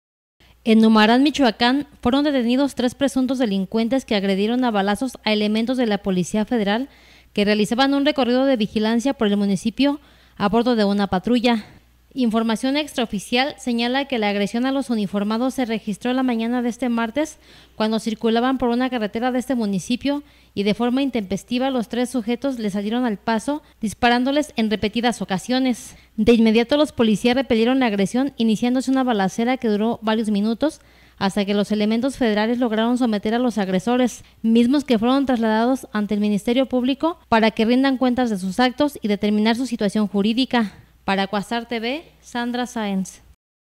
Detenidos en Numarán tres sujetos que agredieron a elementos de la PF
En Numaran, Michoacán, fueron detenidos tres presuntos delincuentes que agredieron a balazos a elementos de la Policía Federal que realizaban un recorrido de vigilancia por el municipio a bordo de una patrulla.